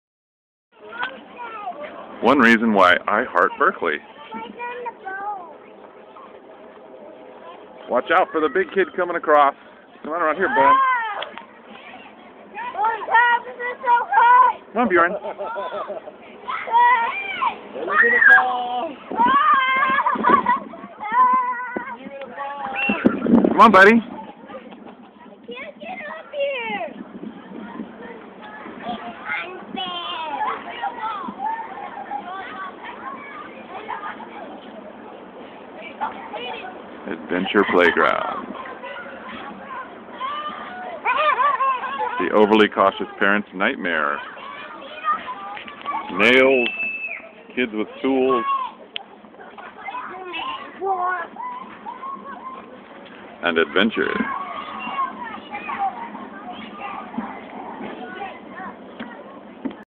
Adventure Playground
A trip to the Adventure Playground in the Berkeley Marina on a beautiful July day